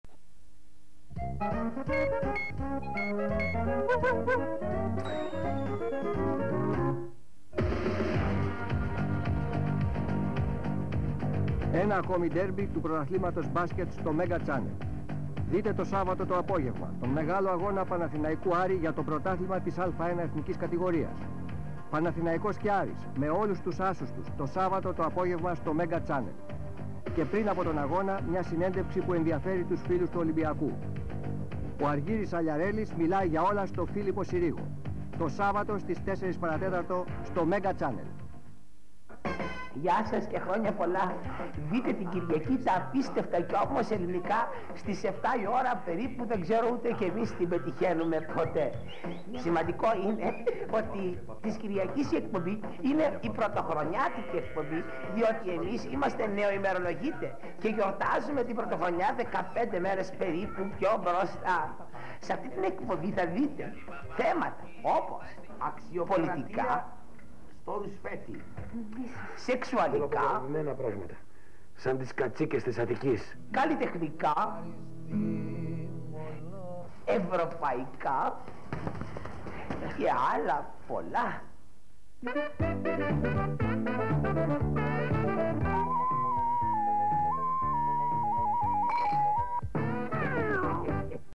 MEGA ΤΡΕΙΛΕΡ
Τρέιλερ του MEGA για το πρόγραμμα του. Μεταδόθηκε τον Δεκέμβριο του 1991.